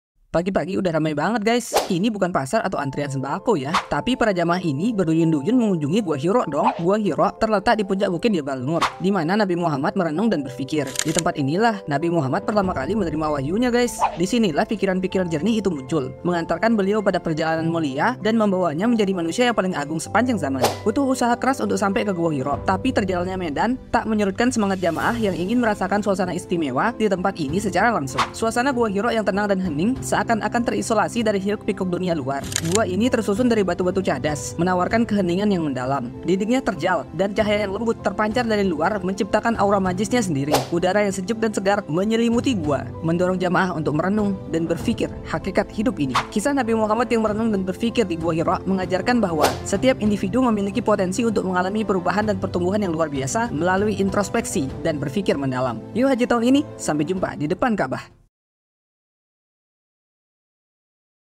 Ramainya Gua Hira
Tempat istimewa itu bernama GUa hira. Sua gua di puncak bukit jabal Nur, menjadi saksi turunnya wayu pertama kepada Nab Muhammad SAW. Jika kamu ingin merasakan keberkahan di tempat ini, maka kunjungilah walau satu kali seumur hidup.
Sejak pagi gua hira begitu ramai, ramai dengan manusia yang ingin mencapai keberkahan yang sama dengan Nabi, untuk menjadi hamba Allah yang dicintai.